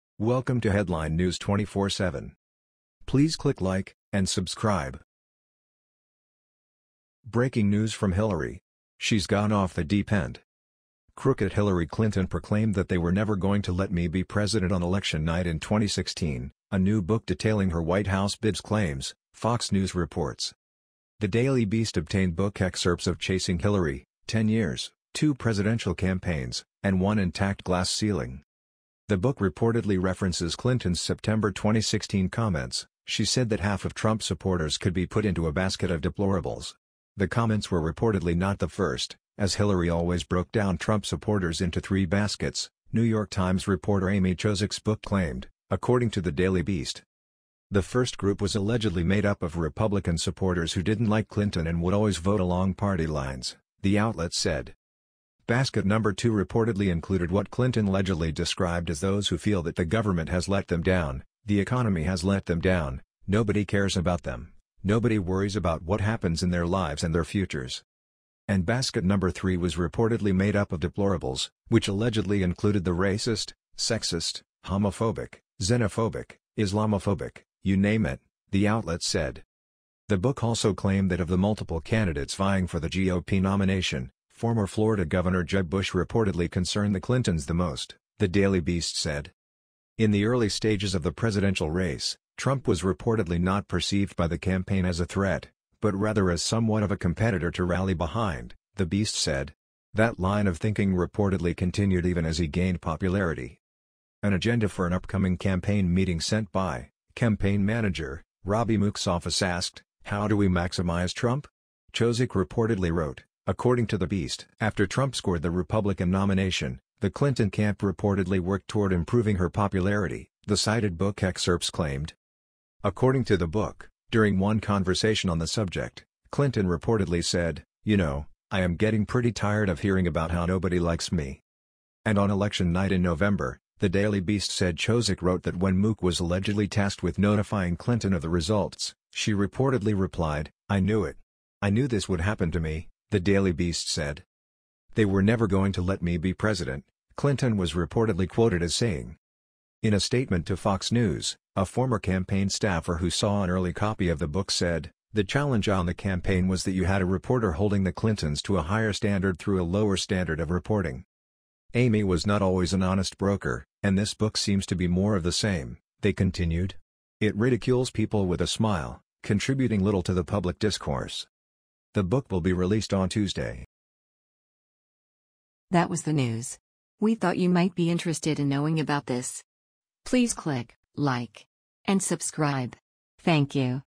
BREAKING NEWS From Hillary… SHE’S GONE OFF THE DEEP END!!!
Crooked Hillary Clinton proclaimed that “they were never going to let me be president” on election night in 2016, a new book detailing her White House bids claims, Fox News reports.